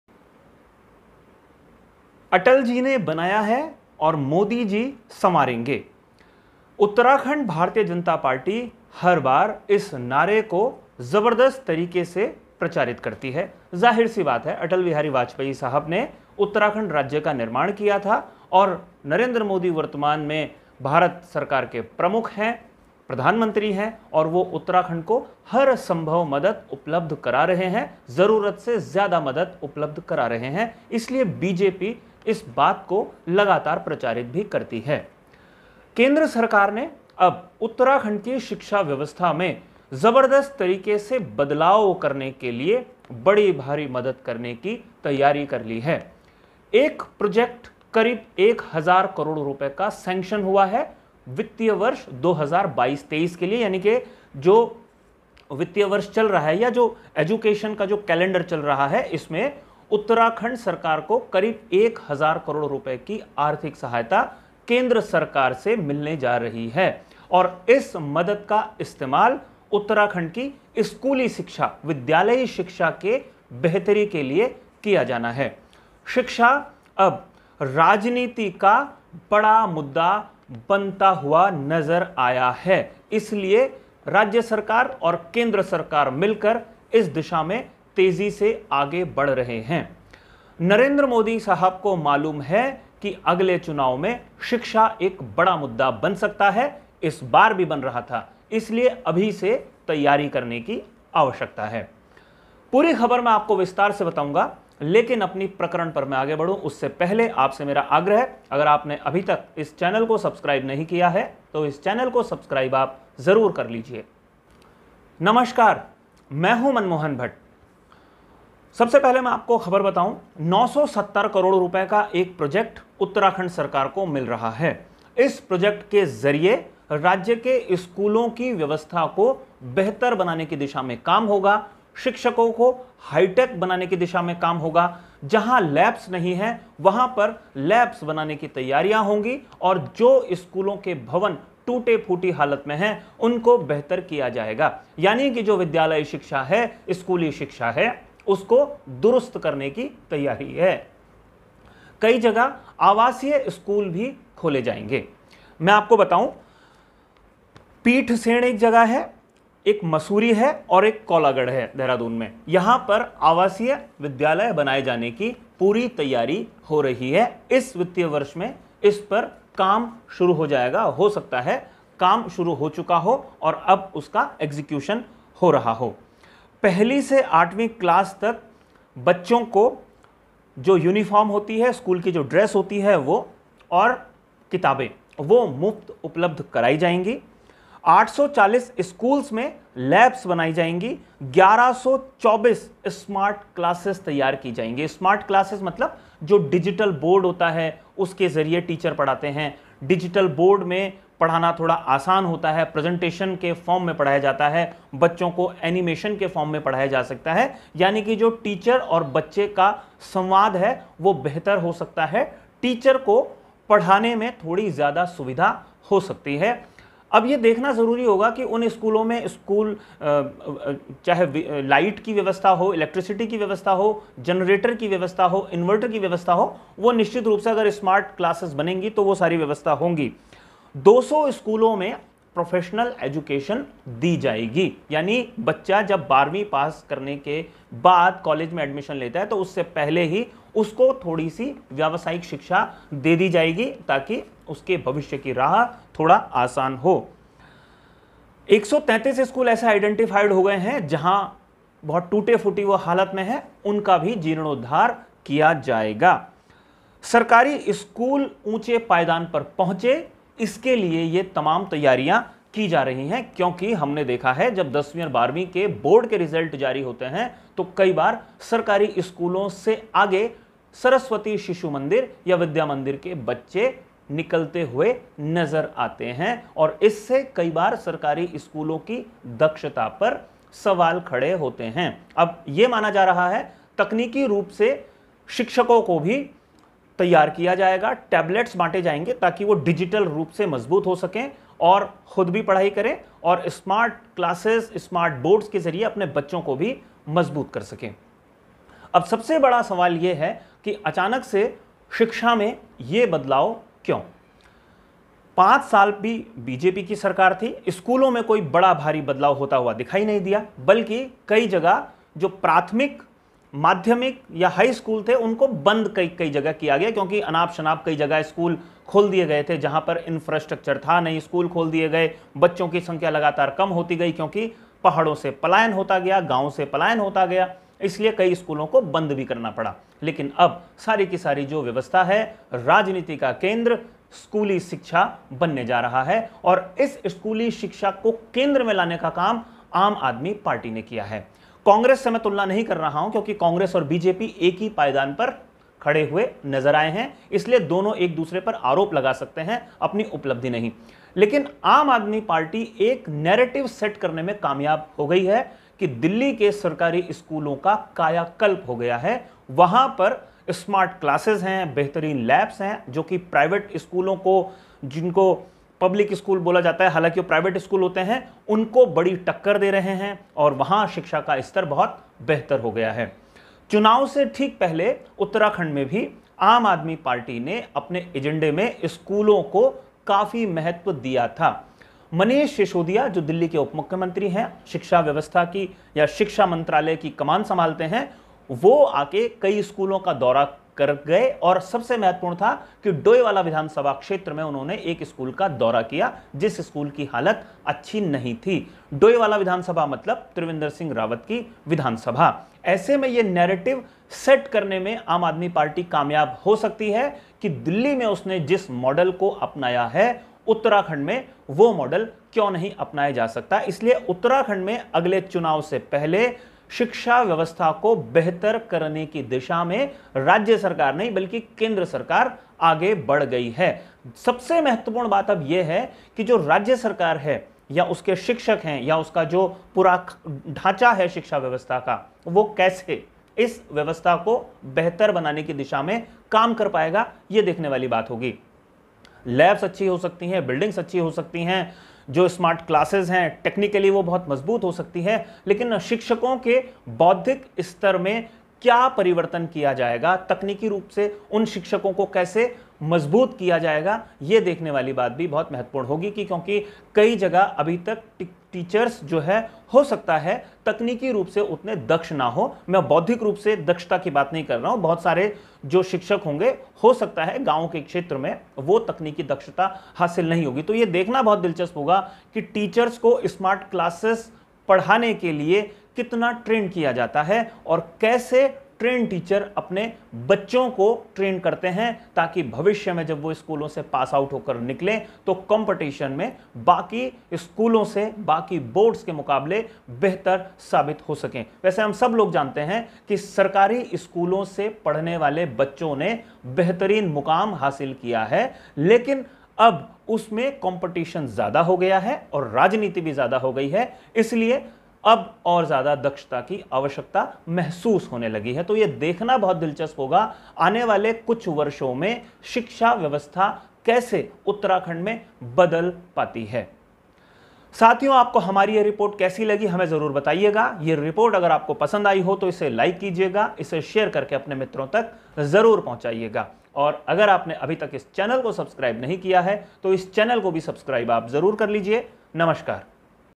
PM नरेंद्र मोदी ने Uttarakhand को फिर दी मदद.शिक्षा में सुधार के लिए मिला प्रोजेक्ट. Manmohan_Bhatt
केंद्र सरकार ने उत्तराखंड के एक और महत्वपूर्ण प्रोजेक्ट को मंजूरी दे दी है। केंद्र ने उत्तराखंड में शिक्षा व्यवस्था को सुधारने के लिए करीब 1000 करोड़ रुपए का प्रोजेक्ट मंजूर किया है।

इस प्रोजेक्ट के मुताबिक उत्तराखंड के सरकारी स्कूलों के शिक्षकों को आधुनिक शिक्षा में दक्ष होने के लिए टेबलेट उपलब्ध कराए जाएंगे। उत्तराखंड के सैकड़ों स्कूलों में हजारों स्मार्ट क्लासेस तैयार की जाएंगी ताकि शिक्षक आधुनिक तरीके से बच्चों को पढ़ा सकें।उत्तराखंड में कई रेजिडेंशियल स्कूल तैयार किए जाएंगे। स्कूलों का जीर्णोद्धार किया जाएगा और कक्षा 1 से लेकर आठवीं तक पढ़ने वाले सभी छात्र छात्राओं को स्कूल ड्रेस के साथ-साथ किताबें उपलब्ध कराई जाएंगी।

 वास्तव में स्कूलों के नाम पर अब राजनीति होने जा रही है और इसमें बीजेपी सरकार बैकफुट पर नजर ना आए इसके लिए तैयारियां अभी से शुरू हो गई है। बीते विधानसभा चुनाव में आम आदमी पार्टी ने उत्तराखंड के स्कूलों को लेकर राज्य सरकार पर बड़ा हमला बोला था । आम आदमी पार्टी ने दिल्ली के स्कूलों की तुलना उत्तराखंड के स्कूलों से करके बीजेपी को बैकफुट पर धकेलने की कोशिश की थी।

 दिल्ली के उपमुख्यमंत्री और शिक्षा विभाग की कमान संभाल रहे मनीष सिसोदिया ने तो तत्कालीन मुख्यमंत्री त्रिवेंद्र सिंह रावत के विधानसभा क्षेत्र में जाकर एक स्कूल का औचक निरीक्षण कर डाला था। इससे कई तरीके की सुर्खियां बनी थी कि मुख्यमंत्री की विधानसभा क्षेत्र के स्कूलों की हालत खस्ता है। अब देखना यह है कि किया है उसका लाभ उत्तराखंड किस तरीके से उत्तराखंड के सरकारी स्कूलों के शिक्षक और छात्र छात्राओं को कितना कर पाते हैं।

देखिए एक रिपोर्ट।